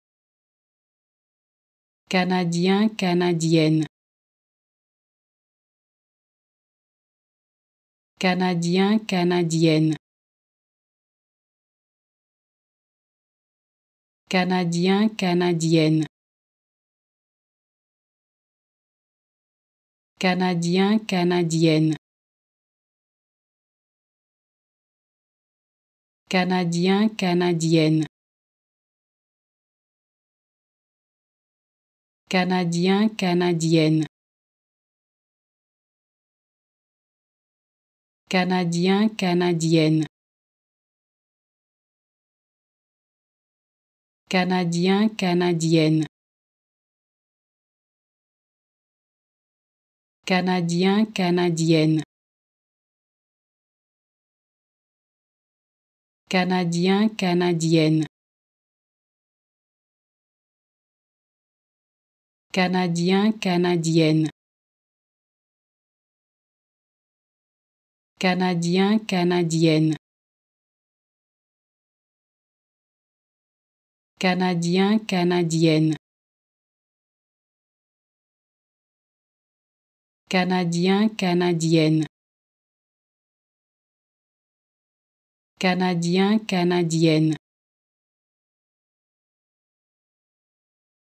How to Pronounce ''Canadien, canadienne'' (Canadian) Correctly in French
Learn how to say and properly pronounce ''Canadien, canadienne'' in French with this free pronunciation tutorial.